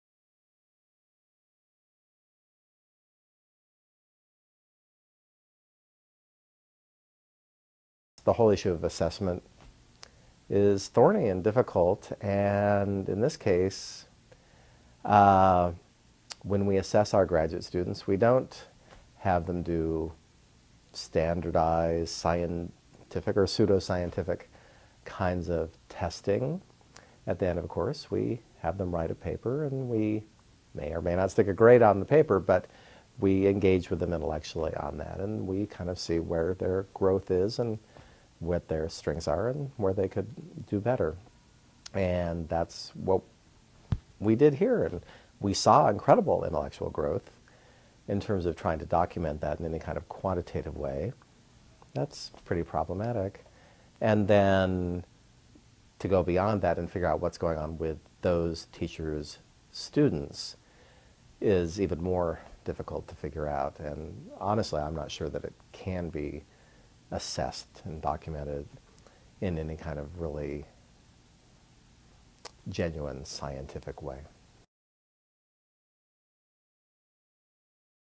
Challenges of Assessment
Robert Johnston describes the high scholarly expectations of a TAH project he participated in, the resulting lesson plans, and the difficulty of assessing project effectiveness.